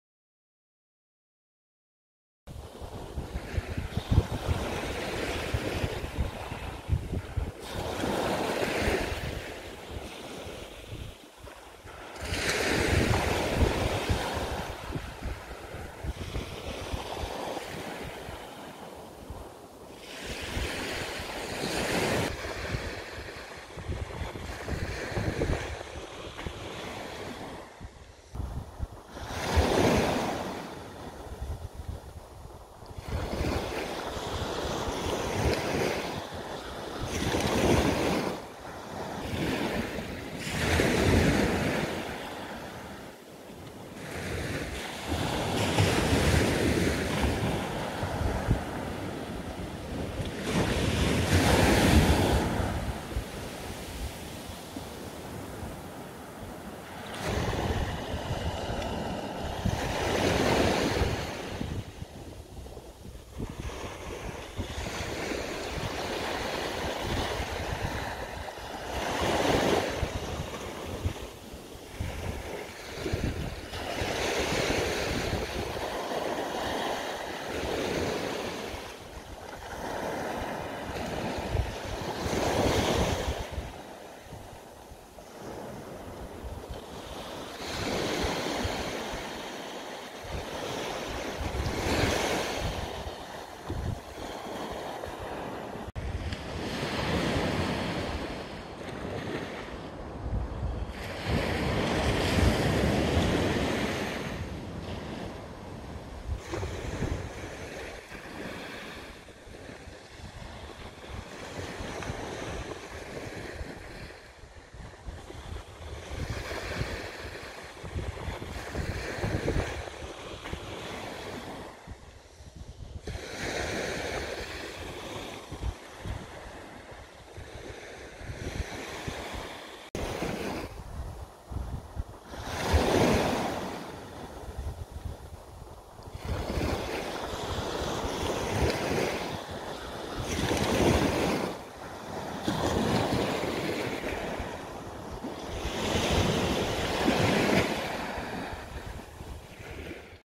Whitestone Branch
On a very dull morning at high tide, I made a beach sculpture.